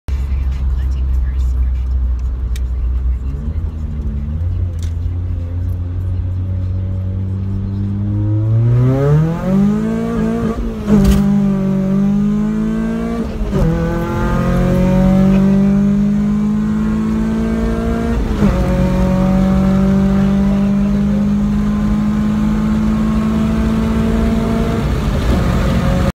Taking my dad on test drive
This is non vtec b20 before I go vtec and turbo😎